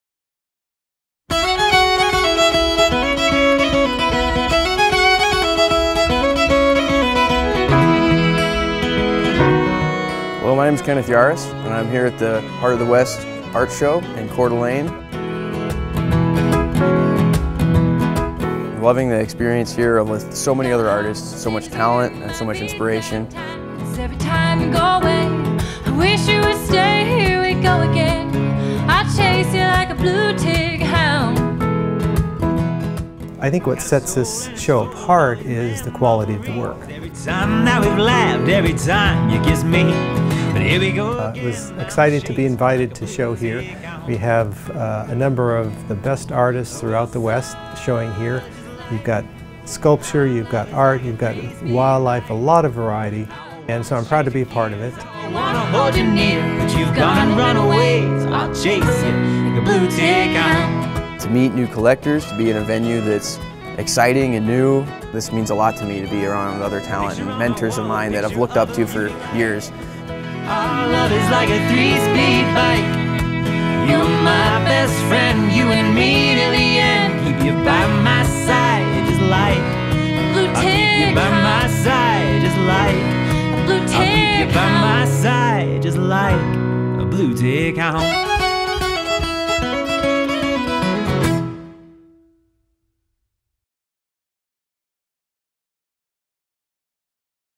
Heart of the West Art Show Coeur d'Alene Highlights
Heart of the West Art Show & Auction is a four-day fine art exhibition and sale at the Coeur d'Alene Resort in Idaho, with the inaugural show taking place Aug. 20-22, 2015.  Future exhibitions will be held Labor Day weekend (Sept. 1-4, 2016) in the Convention Center of the Resort with over 60 premier painters and sculptors of the American West.  The exhibition gives attendees the opportunity to meet and chat with their favorite artists surrounded by a body of work while enjoying demonstrations, educational presentations and a live art auction with ~100 lots.  Join us for the fun next year!